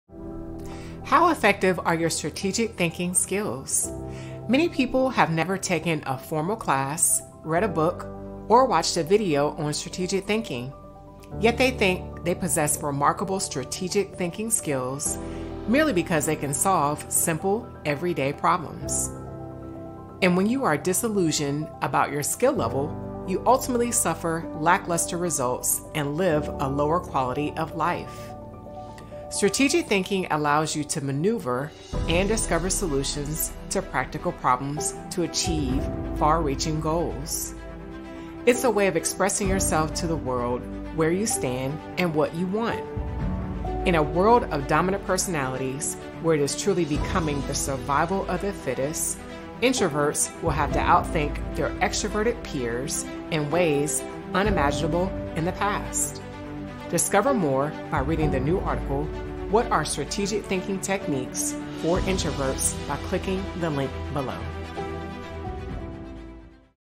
What Are Strategic Thinking Techniques for Introverts?
How effective are your strategic thinking skills?

Many people have never taken a formal class, read a book, or watched a video on strategic thinking. Yet, they think they possess remarkable strategic thinking skills merely because they can solve simple, everyday problems.

And when you are disillusioned about your skill level, you ultimately suffer lackluster results and live a lower quality of life.